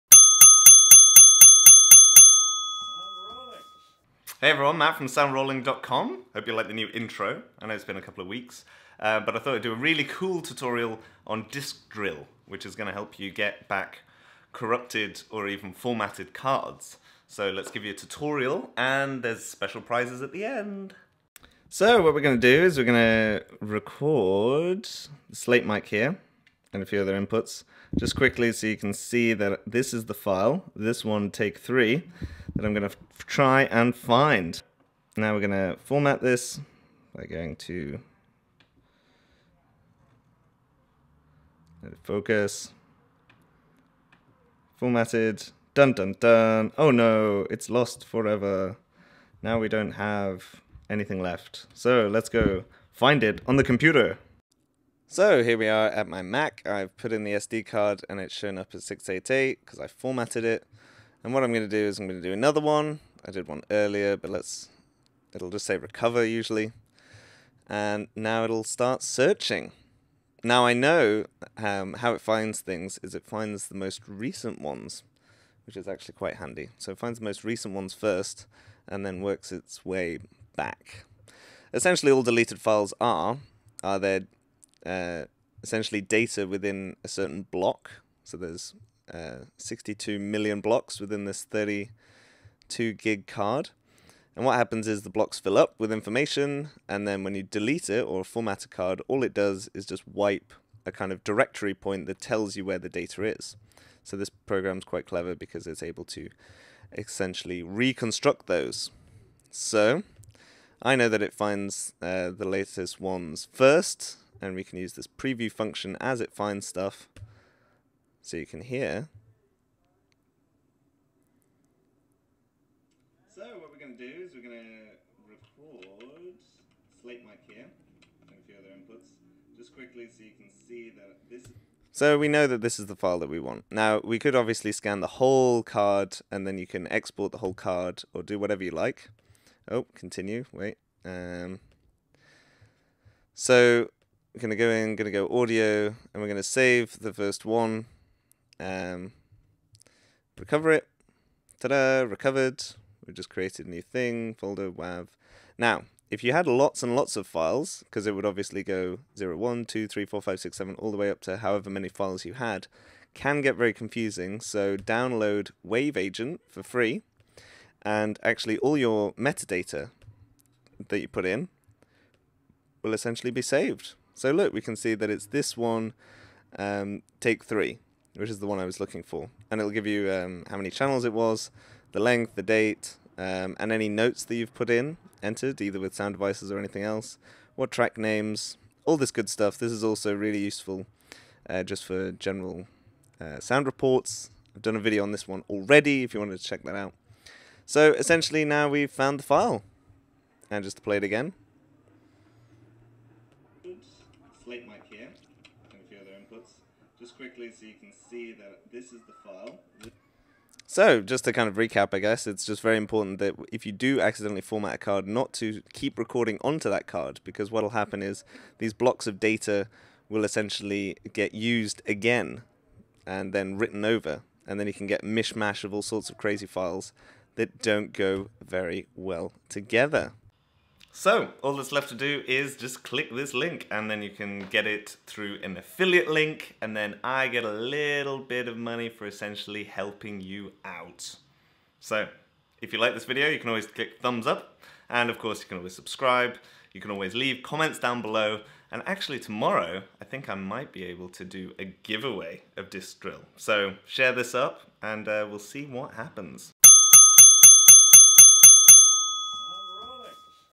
Recover Formatted CF / SD Cards Mac
This is a tutorial of Disk Drill which is £60 and the best and easiest software I have found for recovering not only Sound Files but also any other type of file saved onto an SD or CF card.

About Matt Price